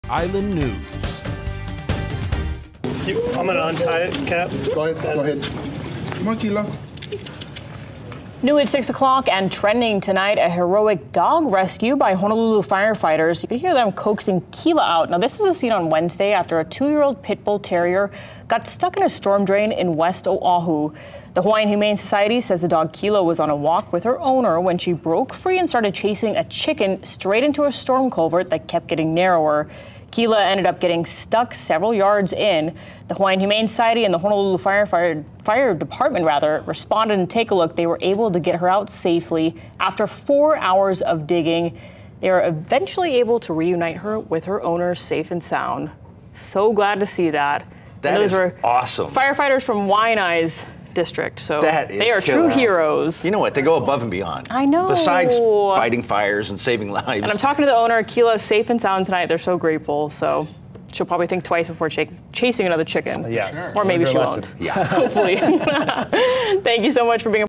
Honolulu firefighters rescue dog from storm drain
Honolulu firefighters are being hailed as heroes after rescuing a puppy that got stuck in a storm drain last week.